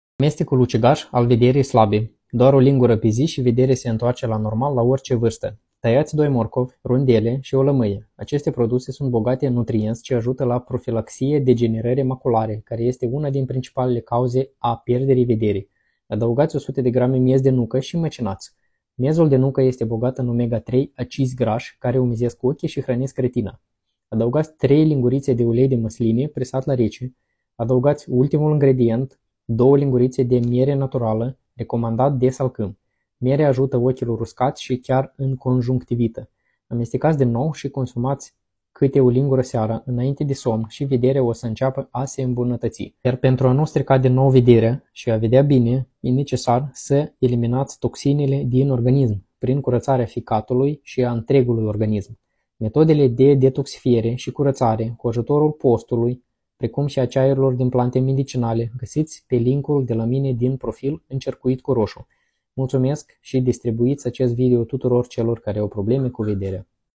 Amestecul natural ce imbunatateste vederea
Acest amestec din cateva componente, va ajuta sa imbunatatiti vederea. Luati cate 1-2 linguri pe zi, seara inainte de somn.